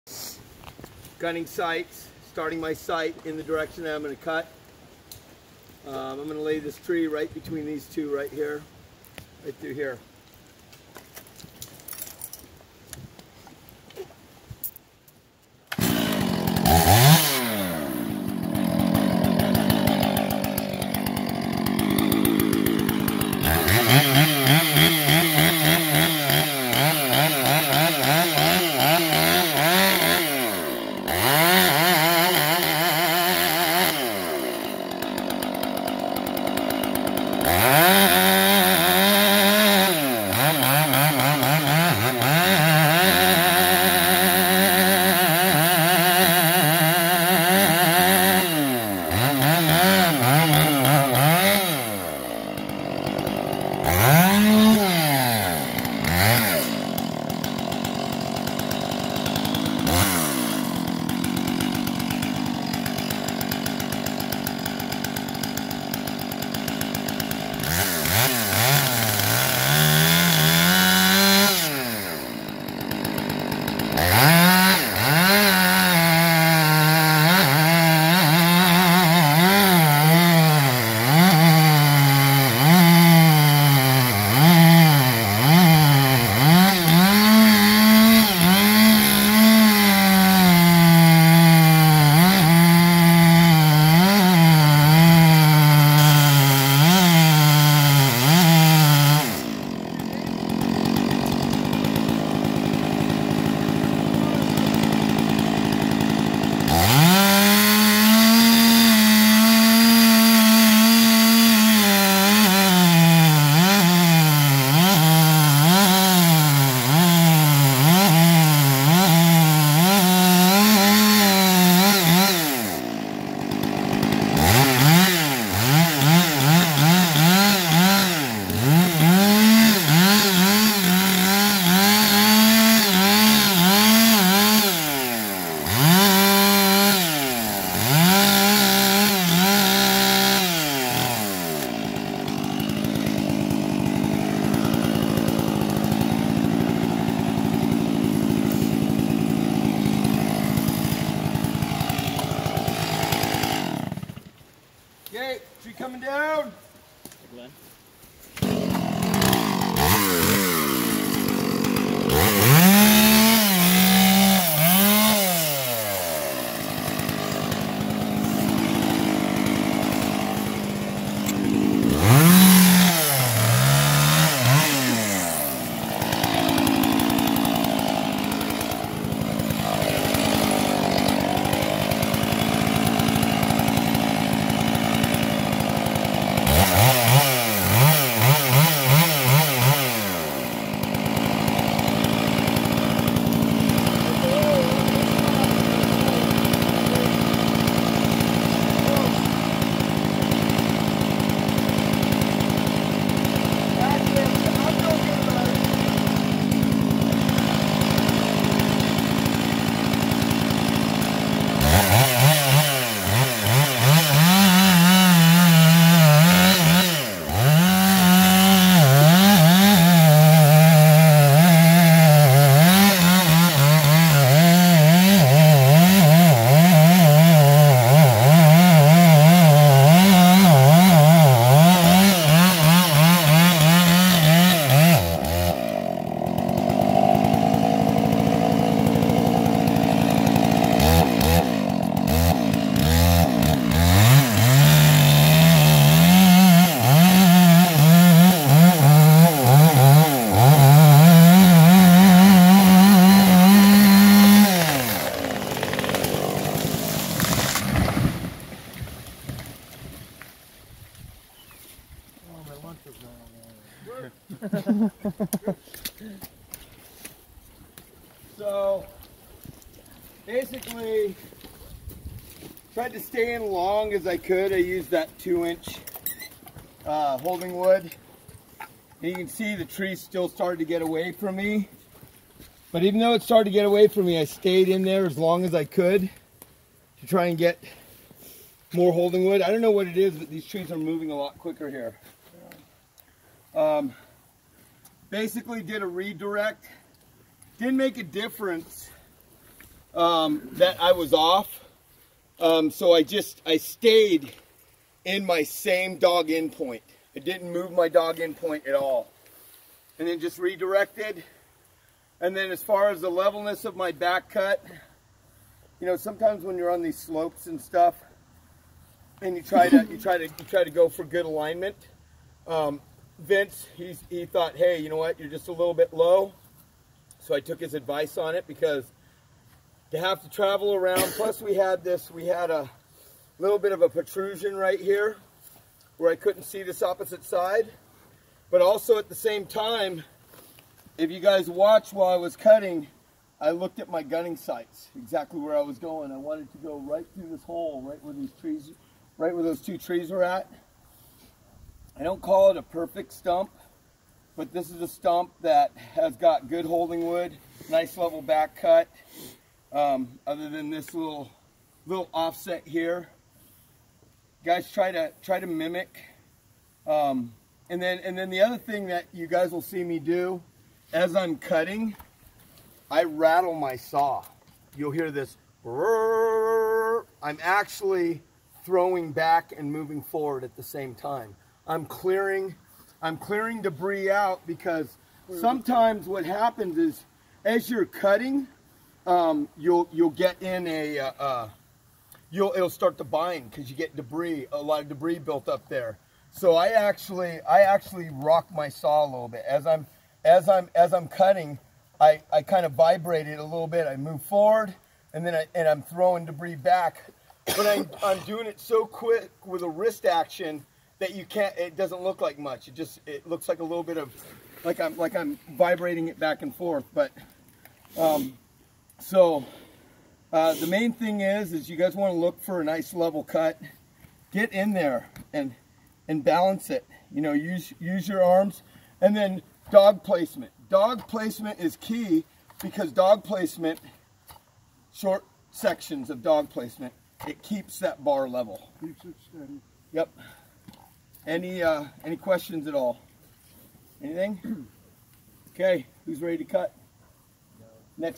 The art of Felling Timber intermediate advanced
Roy giving a tree Felling presentation